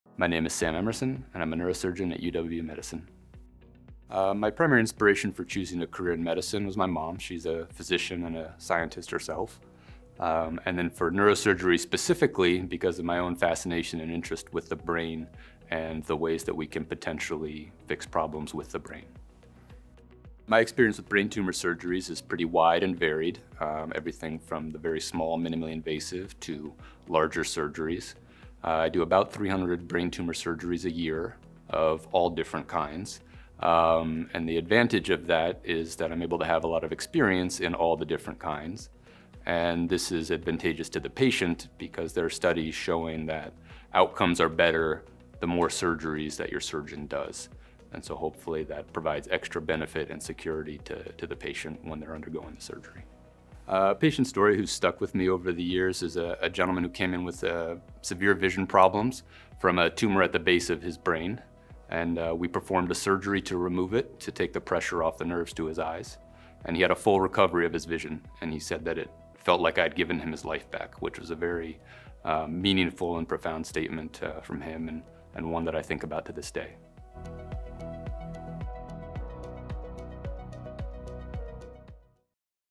Meet Sam Emerson, MD, PhD, Neurological Surgery Care Provider | UW Medicine
UW Medicine Provider Bio
Sam Emerson, MD, PhD
Experienced neurosurgeon specializing in brain tumor surgery, performing ~300 surgeries per year.